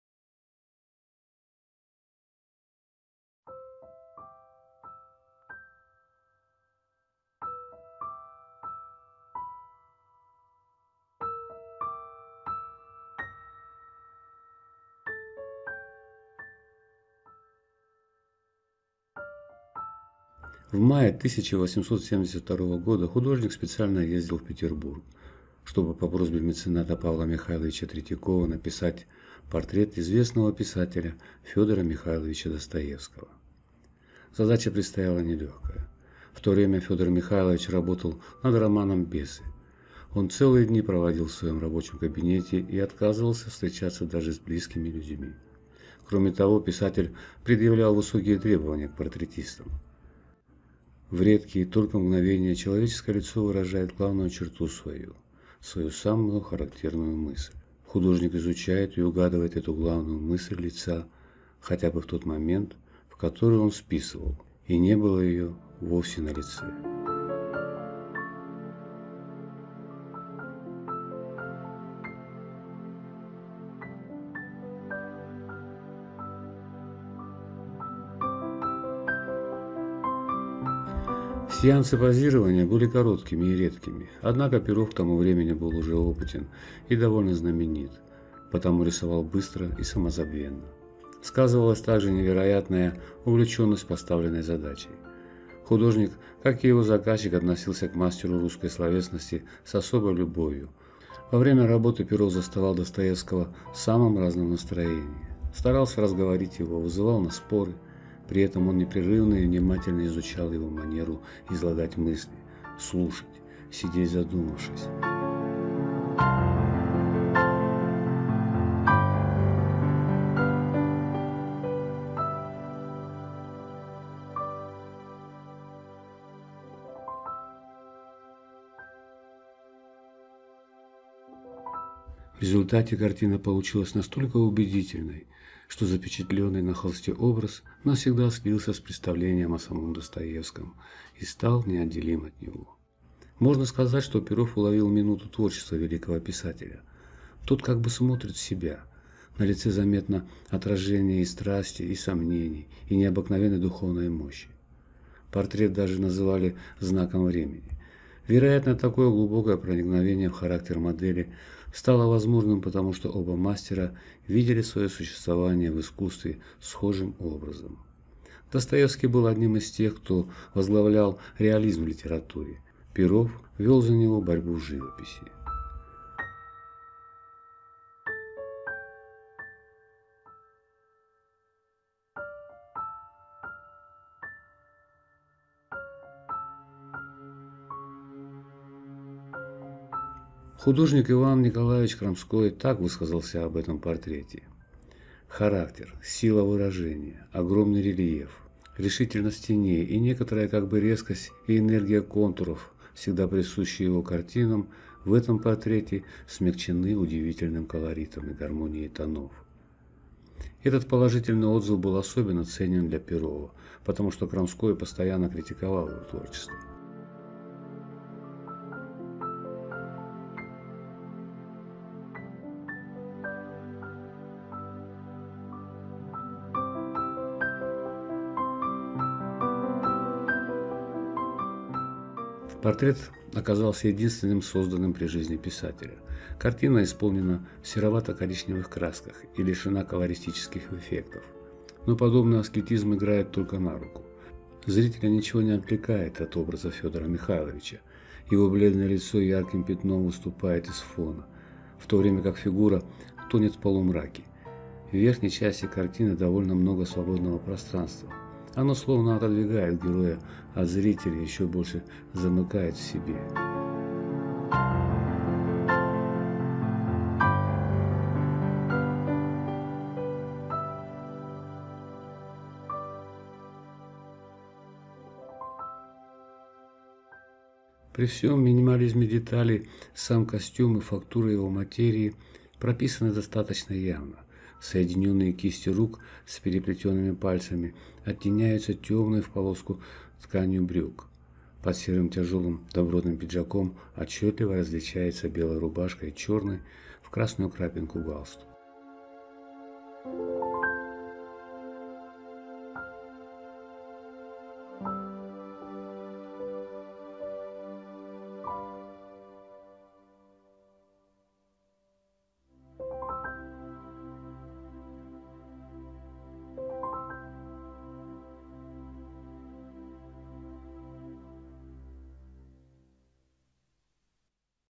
Шедевры русской живописи. Портрет Достоевского.
Весной 1872 года, а точнее в мае, Василий Григорьевич Перов
едет в Петербург, чтобы написать портрет Достоевского.
Причина - просьба Павла Михайловича Третьякова.

 описание картины Портрет Достоевского:
Василий Григорьевич Перов
картина Портрет Достоевского
Дата 1872 г.
техника
масло
холст
Размеры